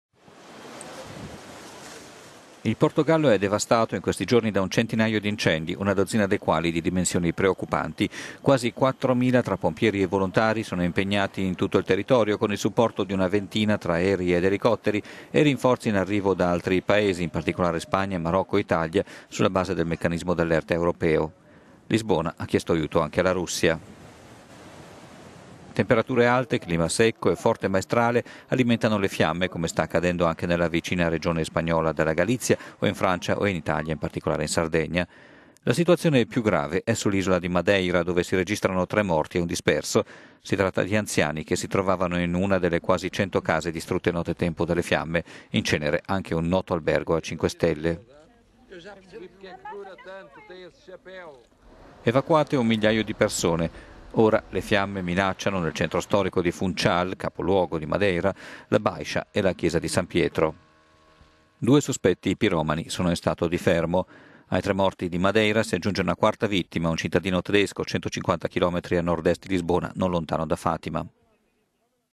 Portogallo: paese devastato da incendi, 4 morti
Il Portogallo è devastato in questi giorni da un centinaio di incendi, una dozzina dei quali di dimensioni preoccupanti. Quasi 4.000 tra pompieri e volontari sono impegnati su tutto il territorio, con il supporto di una ventina tra aerei ed elicotteri e i rinforzi in arrivo da altri Paesi, in particolare Spagna, Marocco e Italia, sulla base del meccanismo d'allerta europeo. Lisbona ha chiesto aiuto anche alla Russia.
Temperature alte, clima secco e forte maestrale alimentano le fiamme, come sta…